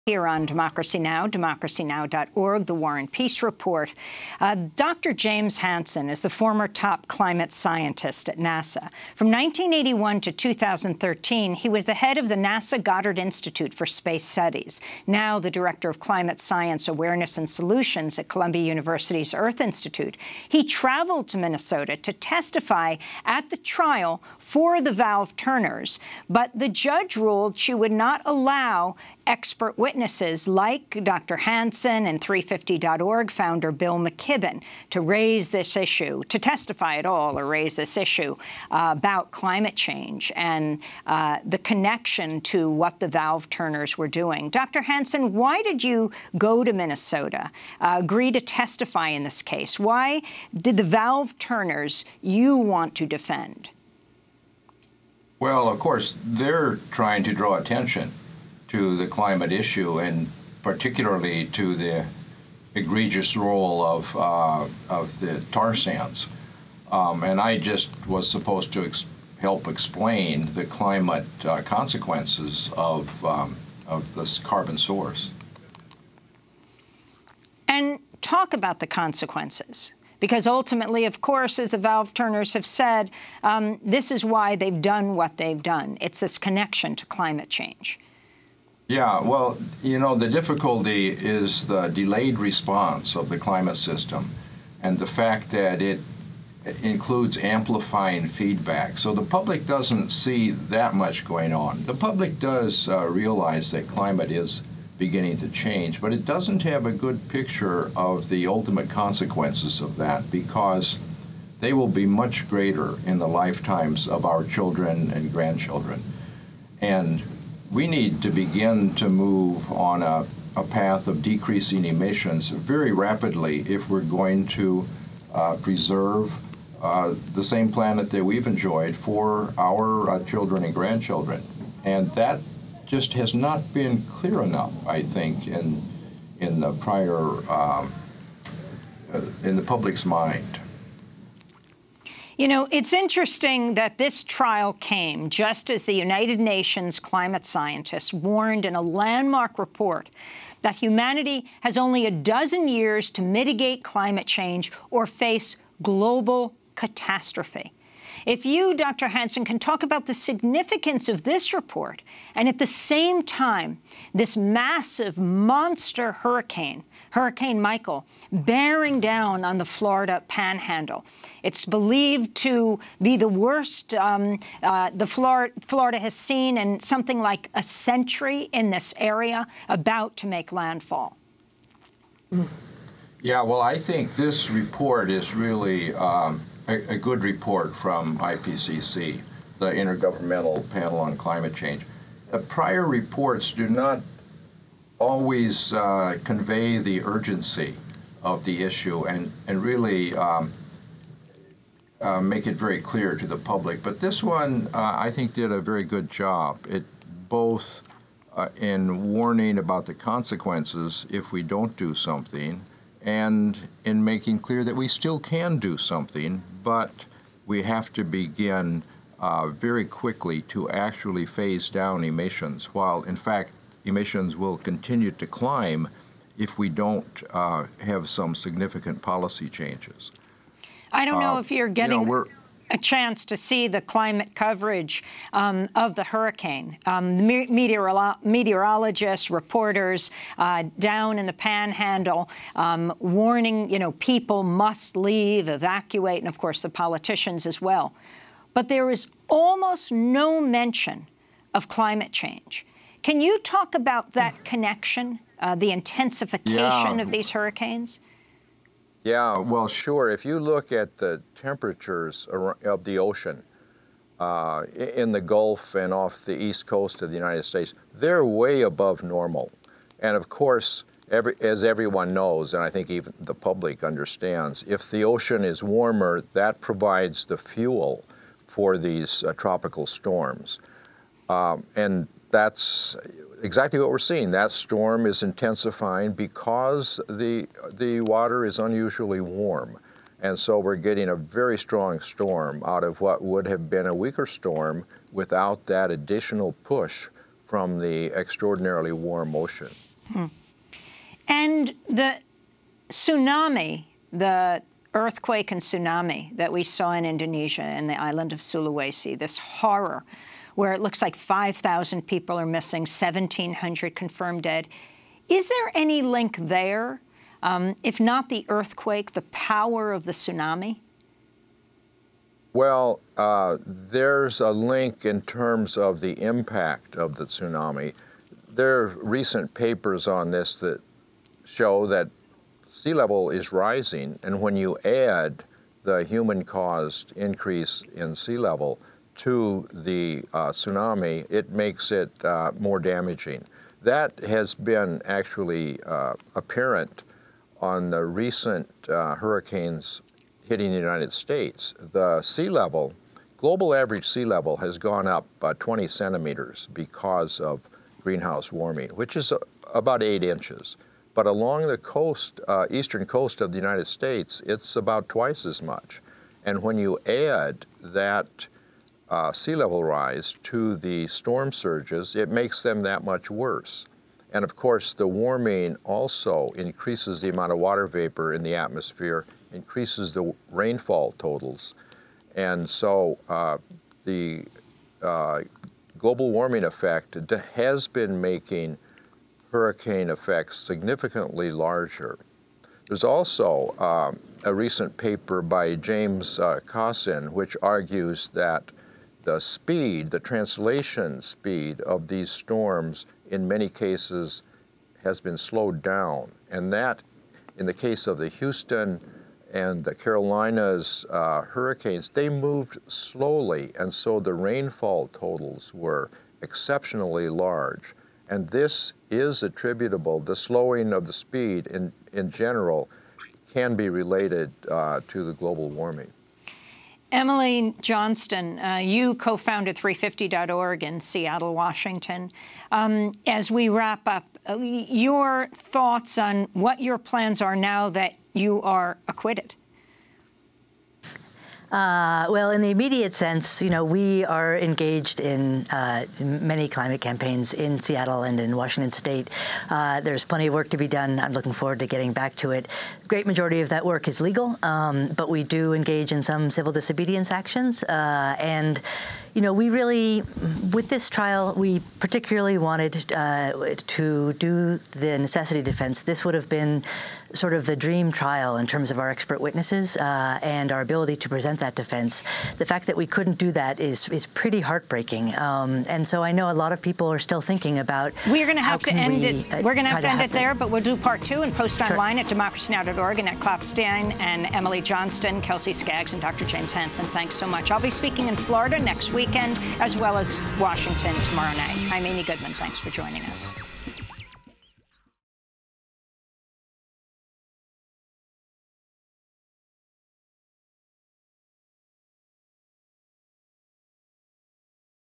Ex-NASA Scientist Dr. James Hansen: We Need to Act Now to Preserve Our Planet For Future Generations
- As Hurricane Michael barrelled down on Florida, famed climate scientist Dr. James Hansen traveled to Minnesota earlier this week to testify on the imminent danger of climate change. He was supposed to be an expert witness at the trial for the “Valve Turners,” the anti-pipeline activists who staged an unprecedented coordinated action to shut down the flow of oil from the United States to Canada in 2016. But the judge ruled she would not allow witnesses like Dr. Hansen to testify on the clear and present danger posed by climate change. On Tuesday, valve turner activists Annette Klapstein and Emily Johnston were acquitted. We speak with Hansen in Minneapolis about the Valve Turners, the recent IPCC report about the imminent threat of climate change and what must be done to save the planet from destruction. Hansen is the former top climate scientist at NASA. He is now the director of climate science awareness and solutions at Columbia University’s Earth Institute. We also speak with Klapstein and Johnston.